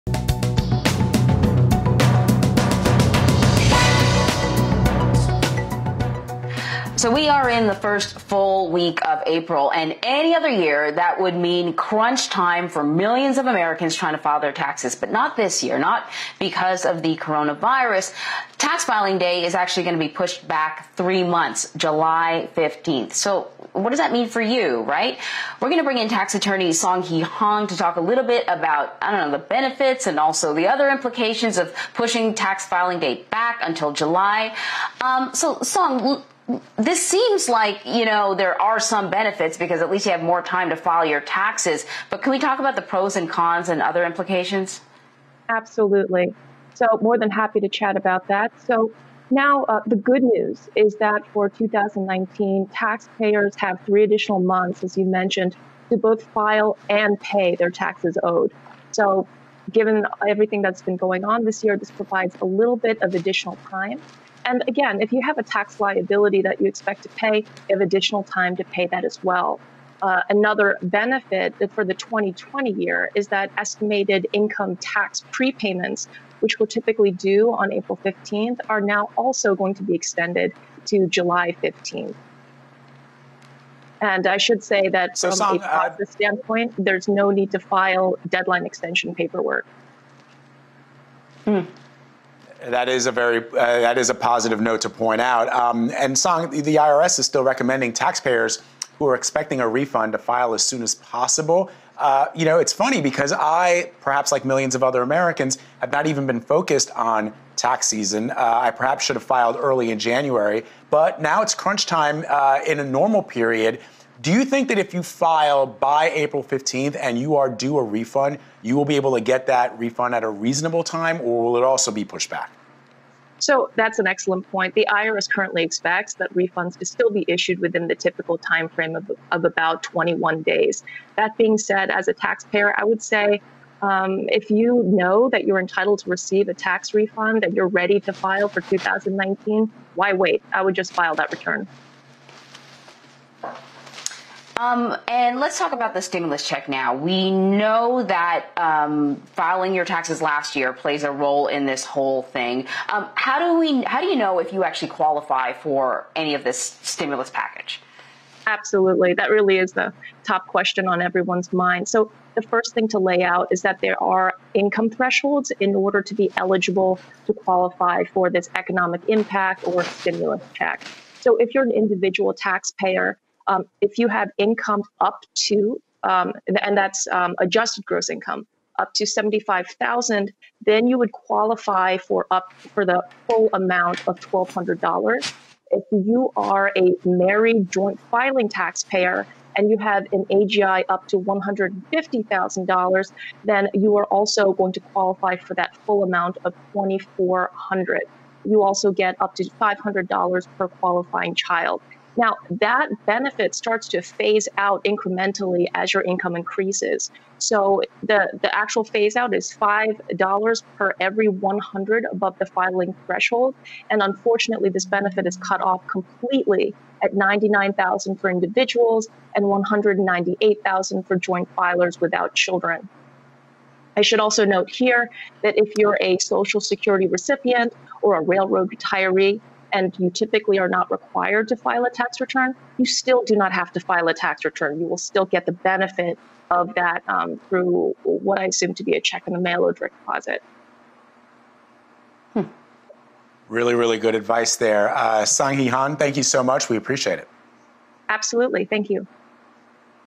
What the 2020 Tax Day deadline extension means for you
In response to the Coronavirus pandemic, the IRS and Treasury Department extended the tax filing and payment deadline from April 15 to July 15.  Tax attorney Sahang-Hee Hahn joins CBSN to discuss the implications of the delay and how it could also impact the anticipated stimulus check.